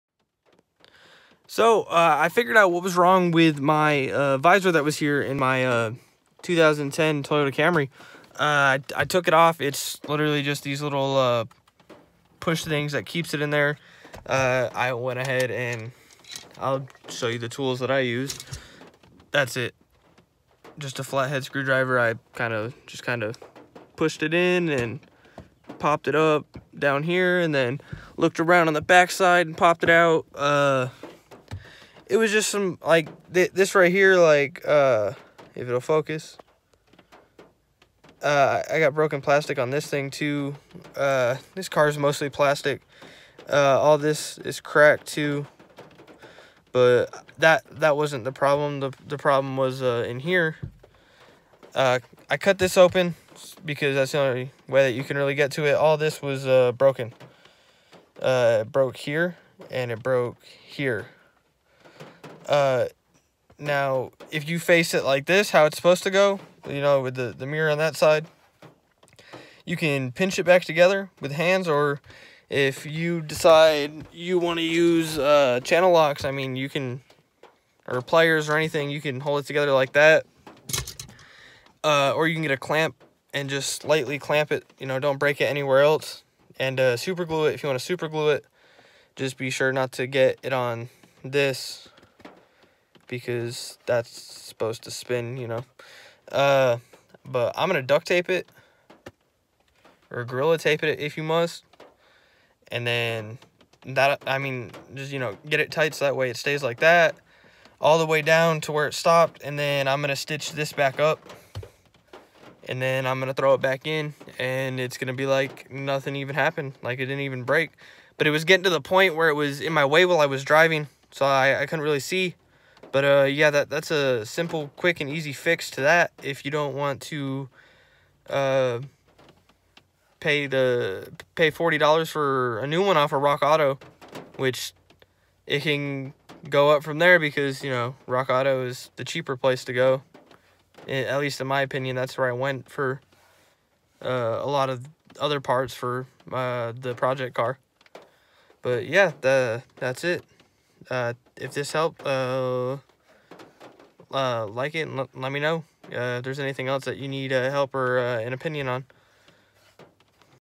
Sun visor on 2010 Toyota Camry not staying upright
Just a quick fix if you don’t wanna spend the money to fix the sun visor or don’t have any extra money like and subscribe and comment if you need advice on anything else related to this vehicle